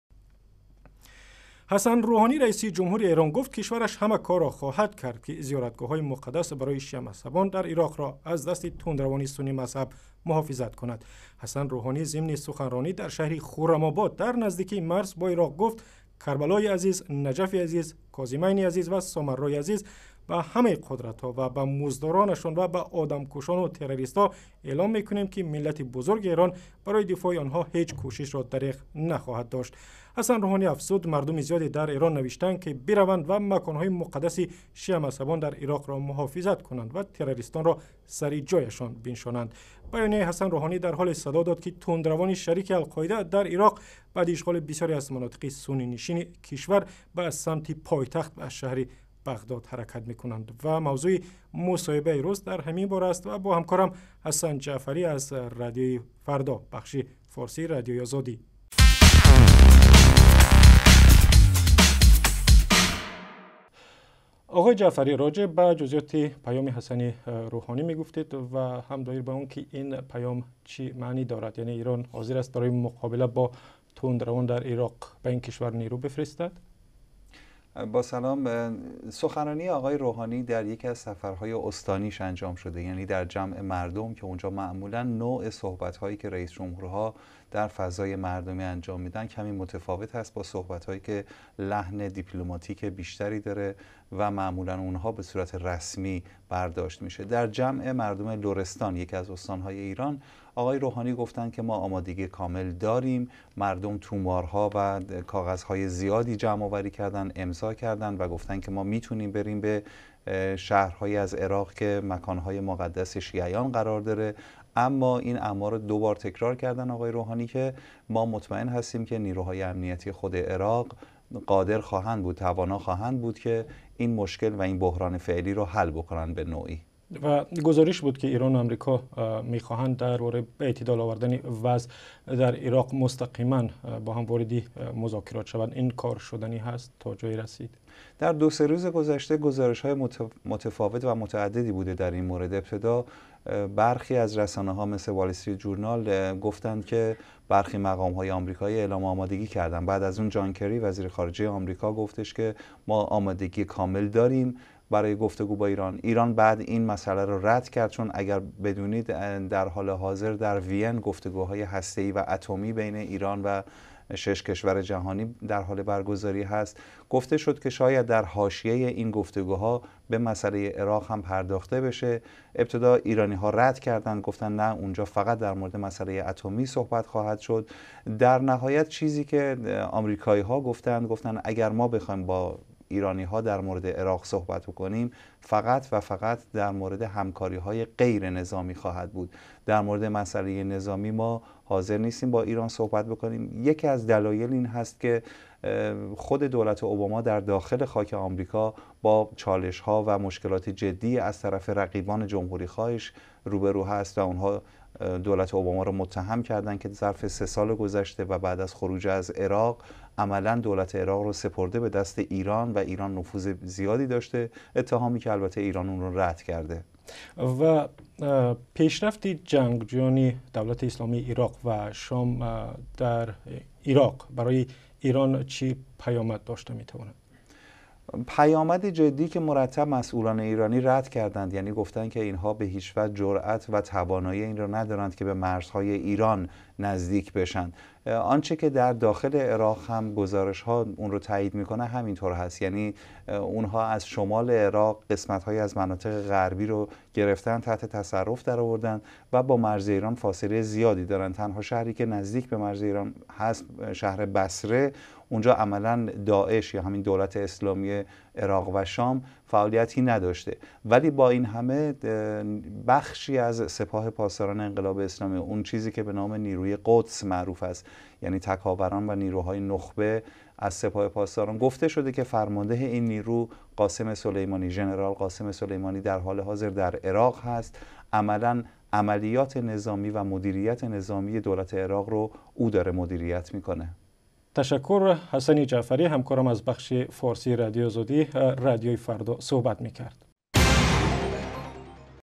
Давлати Эрон мегӯяд, зиёратгоҳҳои шиамазҳабонро муҳофизат мекунад
Ҳасан Рӯҳонӣ, раиси ҷумҳури Эрон гуфт, кишвараш ҳама корро хоҳад кард, ки зиёратгоҳҳои муқаддас барои шиамазҳабон дар Ироқро аз дасти тундравони суннимазҳаб эмин нигаҳ дорад. Ҳасан Рӯҳонӣ зимни суханронӣ дар шаҳри Хуррамобод дар наздикии марз бо Ироқ гуфт, "Карбалои азиз, Наҷафи азиз, Козимайни азиз ва Сомаррои азиз, ба ҳамаи абарқудратҳо ва муздуронашон ва одамкушонаш ва террористҳо эълом мекунем, миллати бузурги Эрон барои дифоъи онҳо ҳеҷ кушишро дареғ нахоҳад кард."
мавзӯи мусоҳибаи рӯз дар ҳамин бора аст ва бо Ҳасан Ҷаъфарӣ аз Радиои Фардо, бахши форсии Радиои Озодӣ.